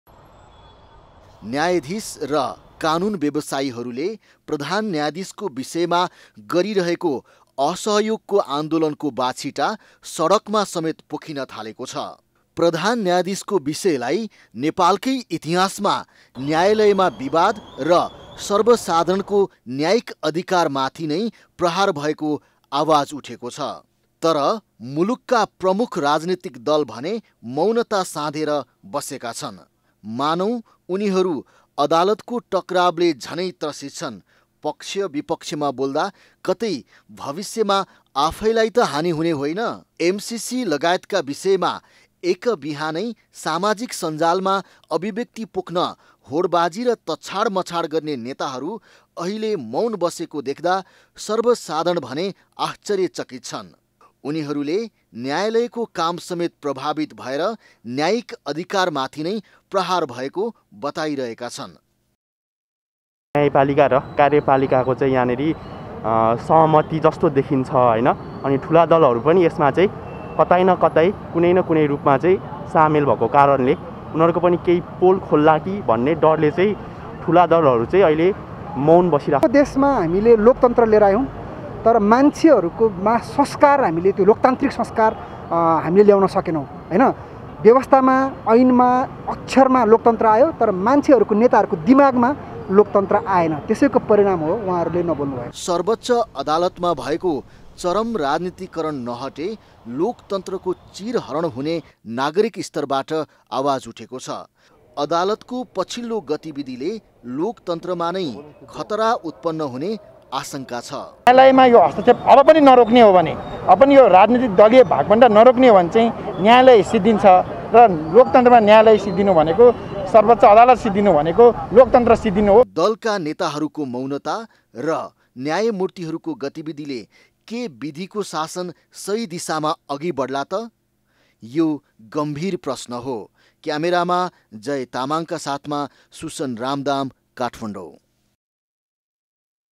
खै न्यायालयको न्याय ?
न्यायालयमा भएको प्रहारका बिषयमा चौतर्फि चर्चा भैरहेका बेला दलहरुको मौनतालाई लिएर टिकाटिप्पणी शुरु भएका छन । न्यायलय भित्र भइरहेका गतिबिधिको पक्ष बिपक्षी आवाज पछिल्लो समय सडकमा समेत पोखिन थालेको छ ।